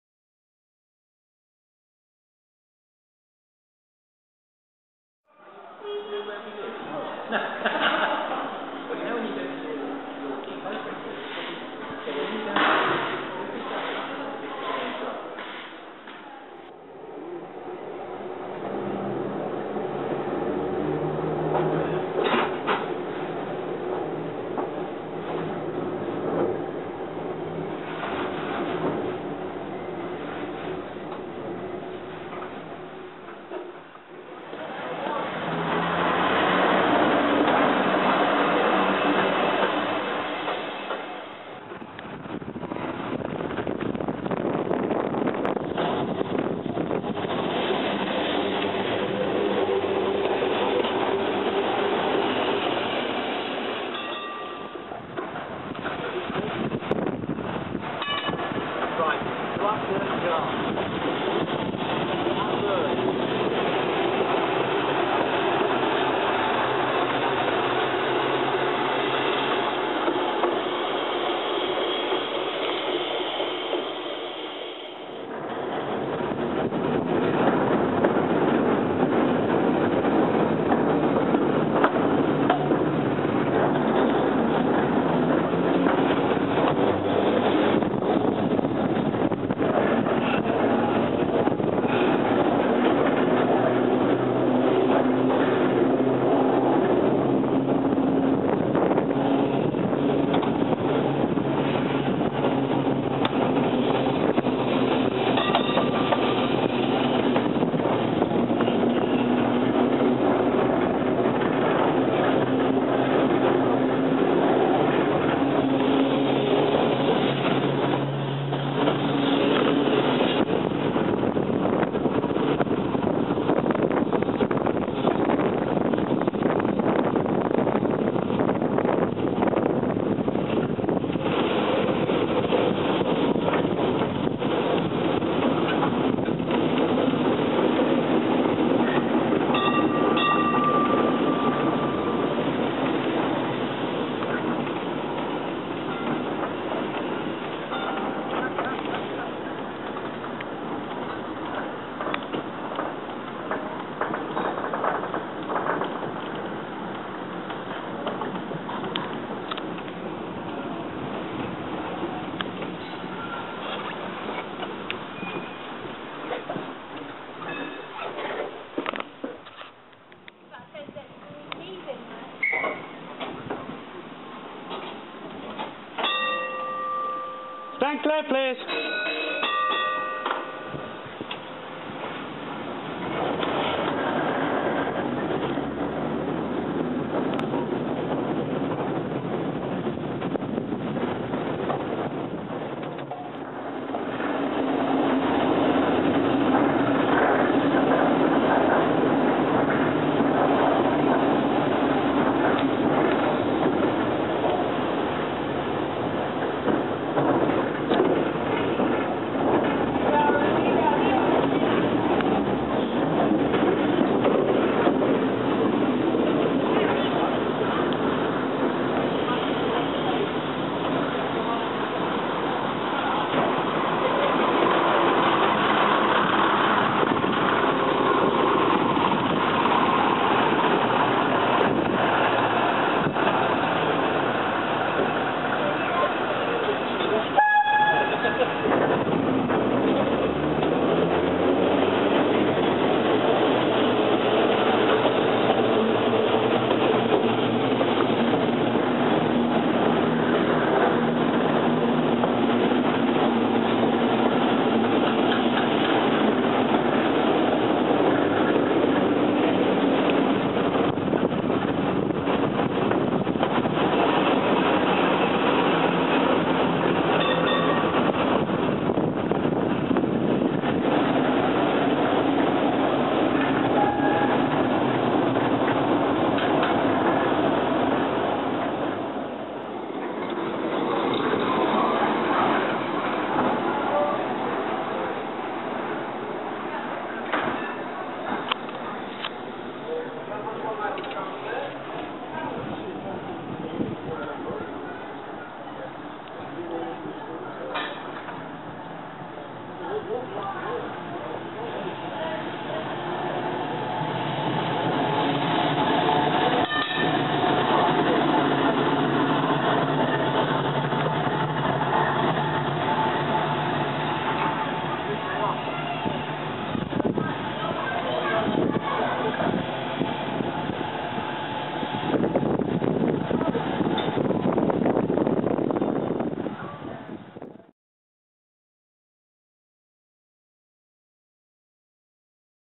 Sheffield 74 (and others): A Conductor's Perspective
A selection of clips from the Enthusiast's Day 2010, shot from the back platform of (and focusing on) Sheffield 74.  Other trams at Crich are seen here, some of which are looked at in my other videos, so check them out!

Sheffield 74 was built in 1900 as an open-topped tram, but just nine years later, in 1909, a top cover was added.  But, not in the usual fashion: Sheffield Corporation Tramways left the platforms and staircases exposed!  This meant, as the trams were out in all weathers, if it rained, snowed, hailed etc then the crew got it all in the face, especially the driver, whilst the passengers sat comfortably indoors.  This gave the tram is unusual look, but it was a very common site in Sheffield in the early 20th Century.
Then, as in every city, the tramways were modernised in the 30s, and Sheffield scrapped the trams like 74 in favour for ones like (Sheffield) 264 and then 510.  Go to Crich to see those! lol
Anyway, 74 was rescued from scrap, and the lower deck came to Crich many years later, the top deck not existing any more, and the Workshop volunteers restored it to the condition you see it in the video.

P.S. After shooting all the footage, we kinda broke the tram.  The north-end circuit breaker broke, so the tram wouldn't move after we parked it for lunch! So myself and my driver ended up crewing on Cardiff 131: the water car, and the first tram to arrive at Crich in 1959